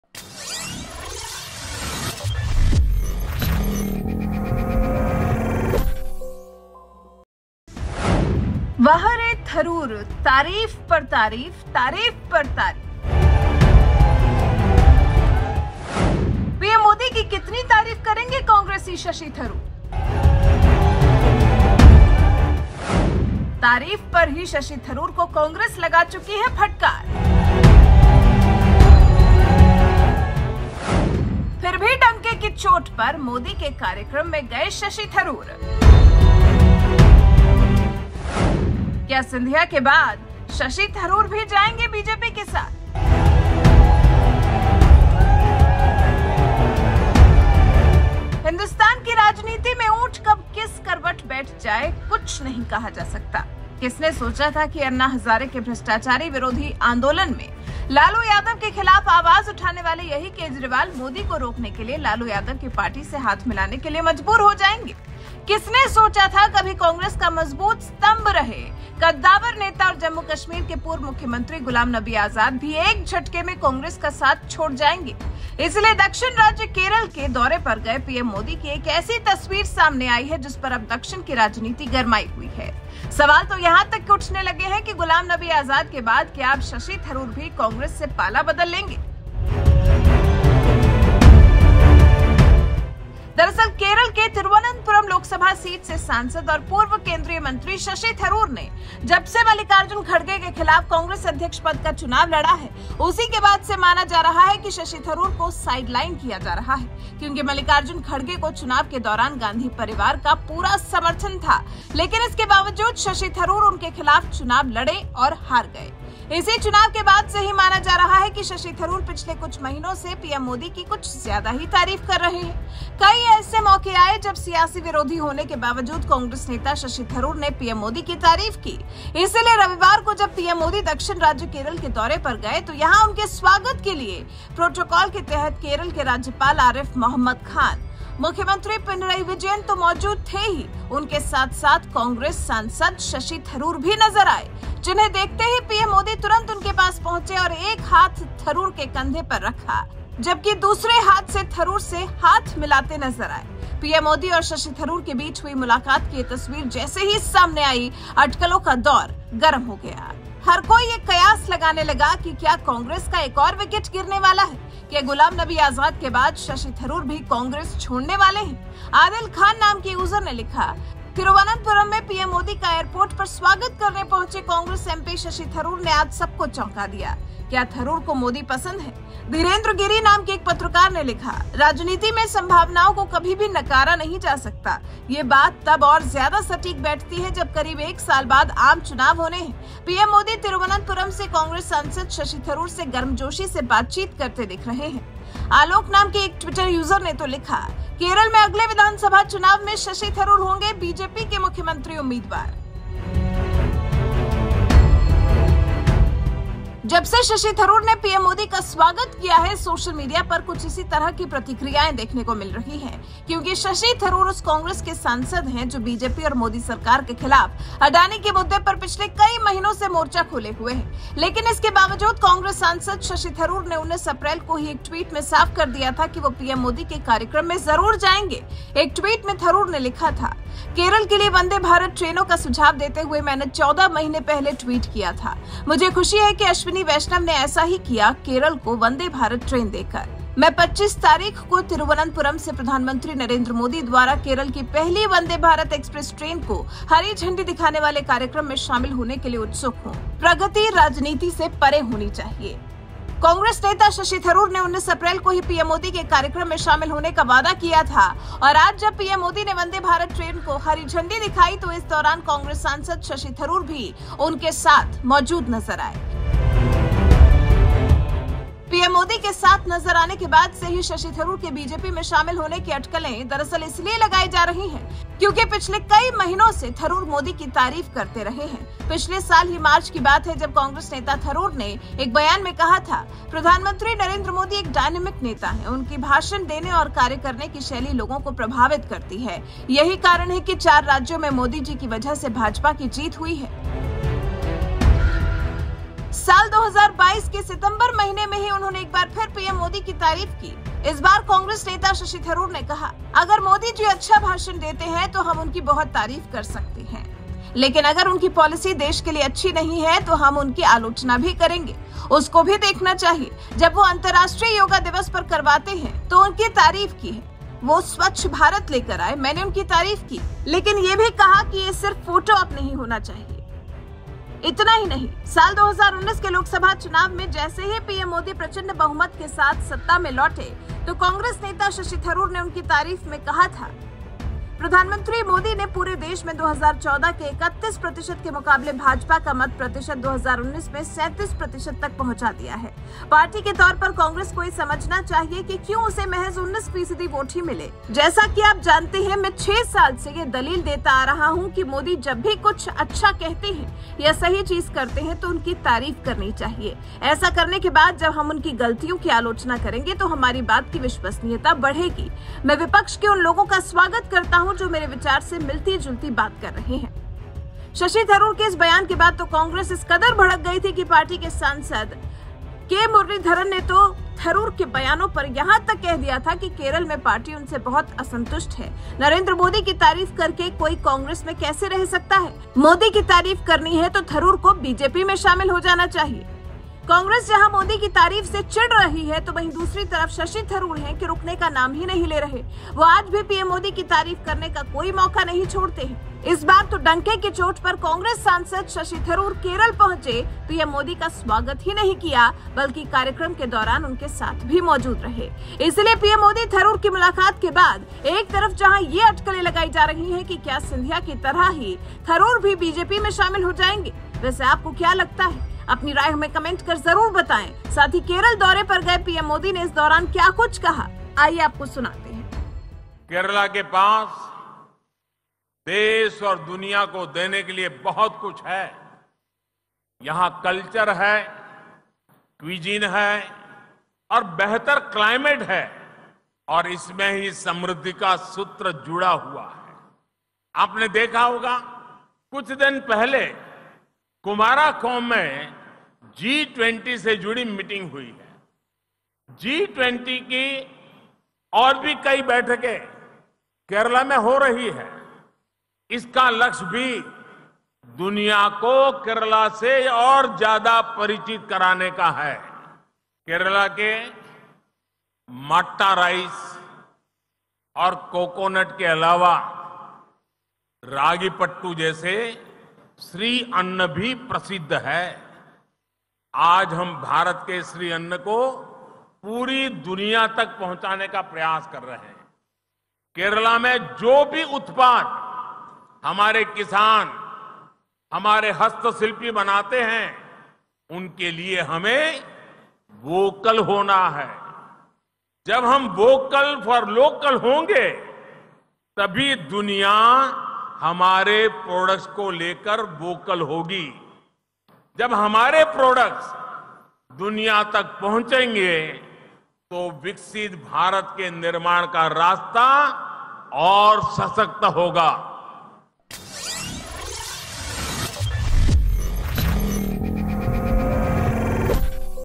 चिढ़ती रही Congress... फिर भी डंके की चोट पर Modi के कार्यक्रम में गये Shashi Tharoor !
दक्षिण राज्य केरल के दौरे पर गये पीएम मोदी की एक ऐसी तस्वीर सामने आई है... जिस पर अब दक्षिण की राजनीति गरमाई हुई है... सवाल तो यहां तक उठने लगे हैं कि गुलाम नबी आजाद के बाद क्या अब शशि थरूर भी कांग्रेस से पाला बदल लेंगे..!

Dheerendra
Neha